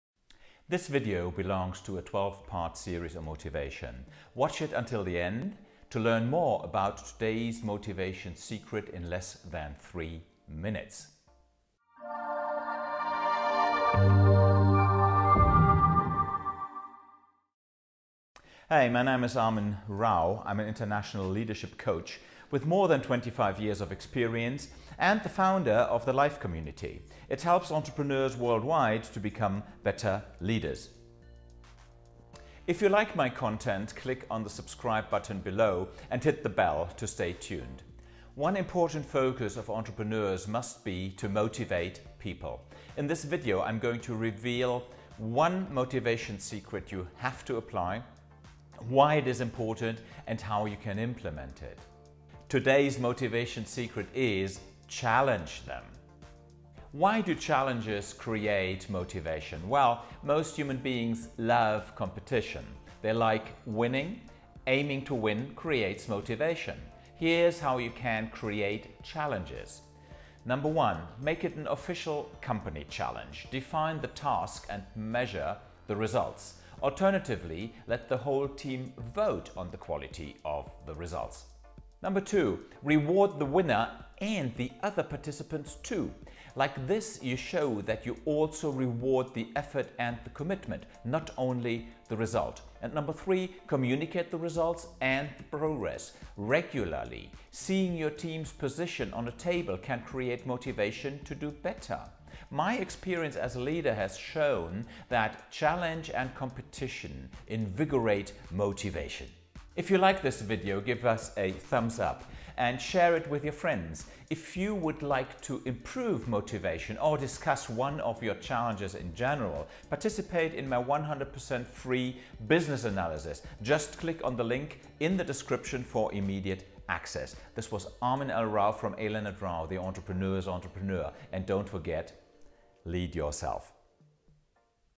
#12 Team-Motivation: How to Keep Your Team Motivated (in under 3 Minutes)
100% free business analysis - I'll share with you my experience and boost your business:

In this video I am going to show you how to keep your team motivated - in under 3 minutes. If you follow my short tips your team motivation will be at a higher level!